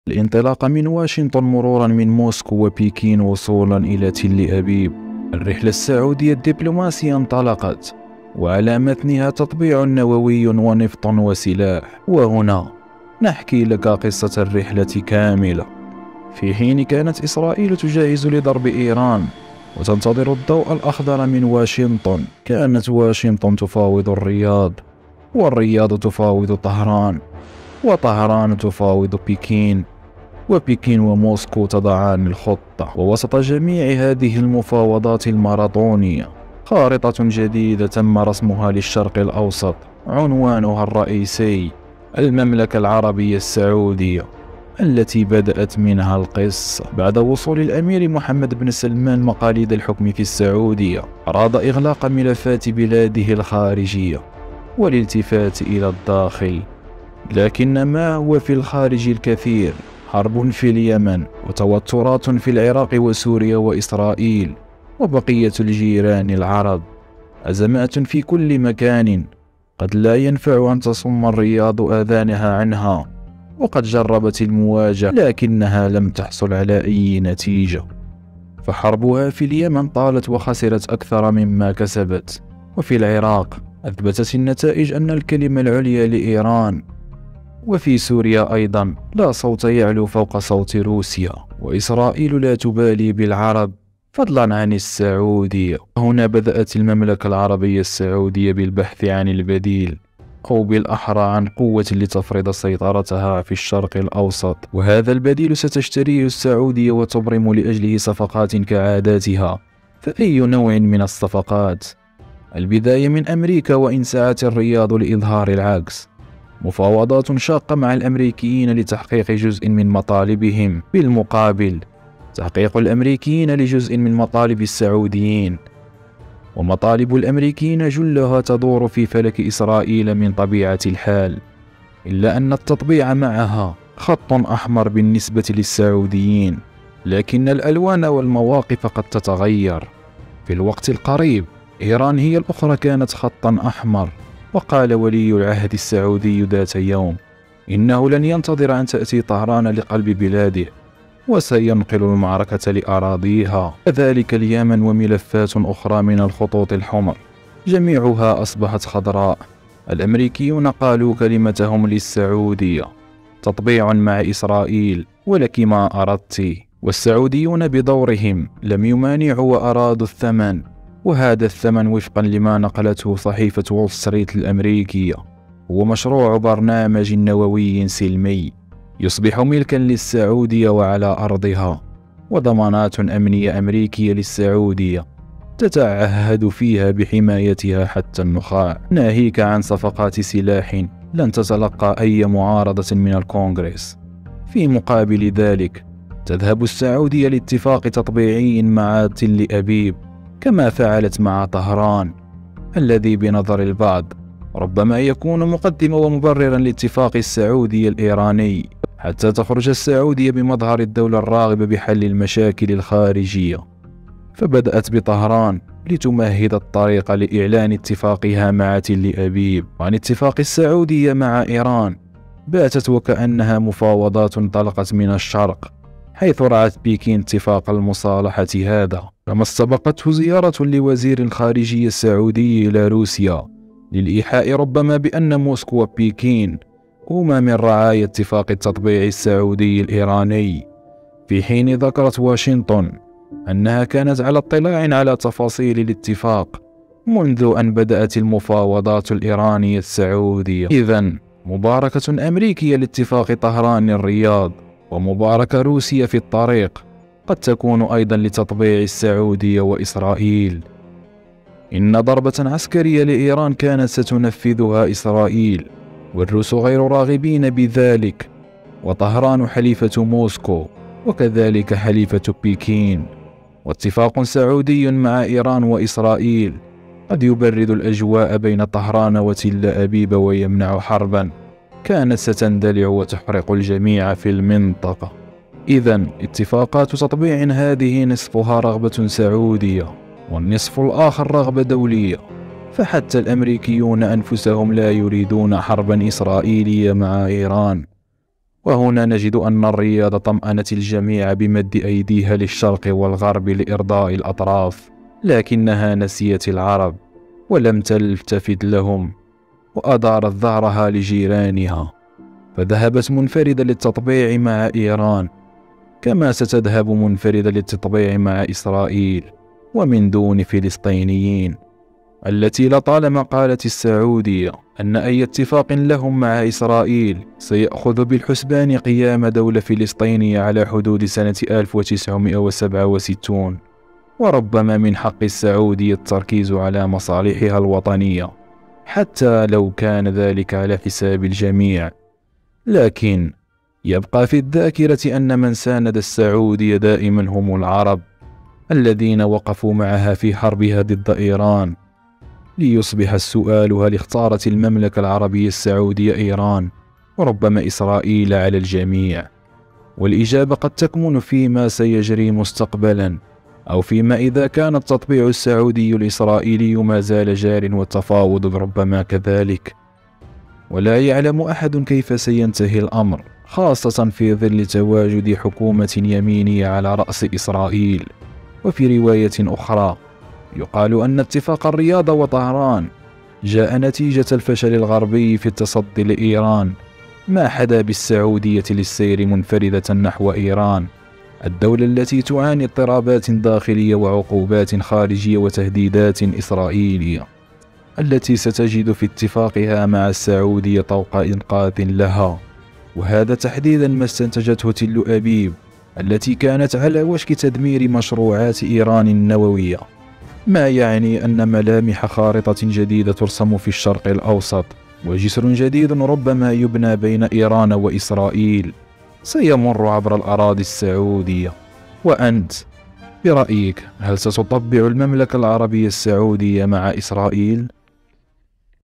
السعودية تتفق مع إيران ... و تفاوض إسرائيل مقابل النووي و الأسلحة
انطلق قطار السعودية من  جديد يحمل على متنه الكثير من الاتفاقات السلمية والمشاريع النووية والسلاح، مر بروسيا والصين وأمريكا وإيران وربما تكون وجهته الأخيرة  إسرائيل،  سلام سعودي إسرائيلي مقابل النووي بعد السلام السعودي الإيراني وهنا نحكي لك القصة والتفاصيل. في الفيديو